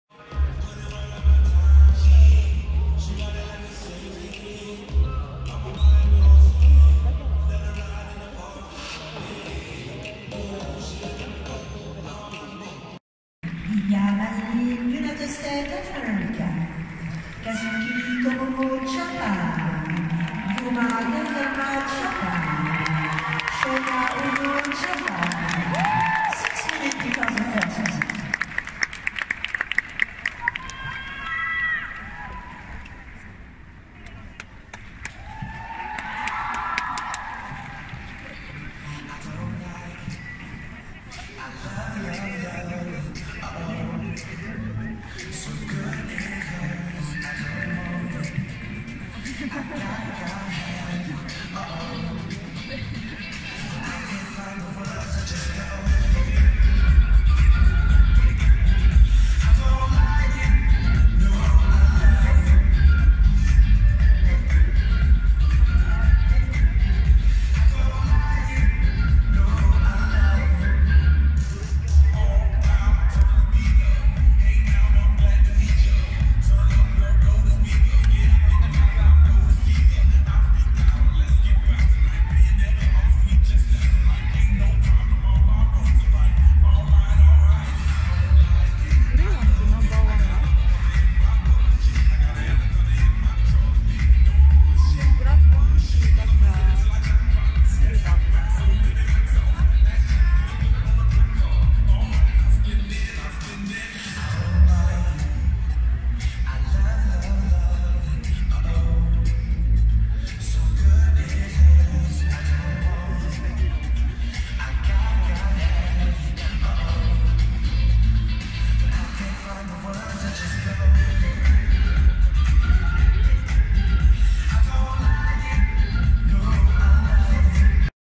【⛸ ＃2 友野/宇野/鍵山3 選手 トレーニング登場 ｜ Three Japanese skaters showed up for Figure Skate Training】
＊2022年モンペリエ フィギアスケート世界選手権 ｜ 公式トレーニング ｜2022 Figure Skating World Championships＊

- 🙇最初に、すみません。わたしは機械音痴で、さらにフィギア観戦初でしたから、ビデオの質が悪いです。気をつけてご視聴ください。

- モンペリエ フィギアスケート世界選手権の公式練習の様子のビデオです。
- 動画アップの順番が入違いましたが、友野一希選手、宇野選手、鍵山選手の登場シーンです💗
- 会場の雰囲気を感じ取ってもらいたいので、音楽はそのままで アップします。
- もしチャンスがあったら、ぜひ公式練習から、フィギアスケートの大会を観戦してみてください。きっと楽しいはずです(^_-)-☆

- I am not good at technology things, so I am very sorry in advance that the video is not good quality.
- This video is the scene when the three Japanese skaters, Tomono Kazuki, Uno Shoma, Kagiyama Yuma, showed up!
- Watching the official training was "first come first serve", so that we could see the skaters so close! If you have chance, please watch the competitions from the training.